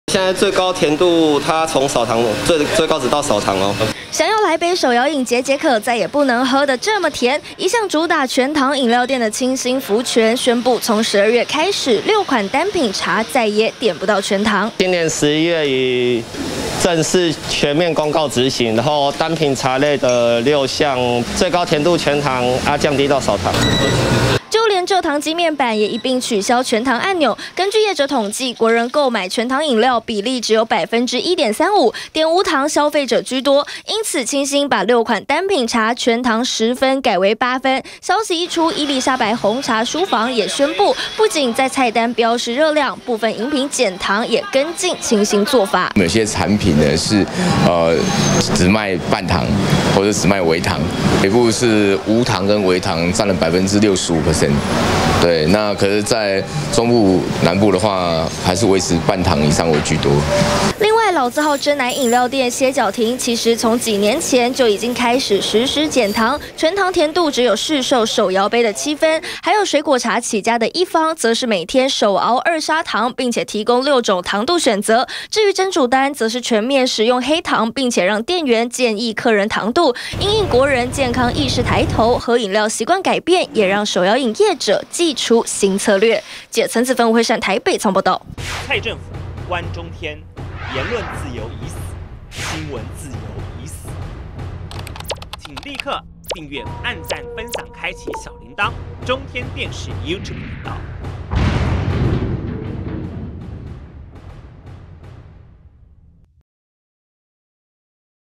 20201202中天新聞 跟全糖說掰掰！ 清心福全12月起6款茶最多「8分糖」
為了響應減糖政策，手搖飲連鎖品牌清心福全，宣布12月開始六款單品茶，甜度從10分改成8分，不少知名手搖飲料店跟進，歇腳亭就說，實施減糖好幾年了，甜度只有一般市售手搖杯7分，伊莉莎白紅茶書房，也宣布跟進減糖。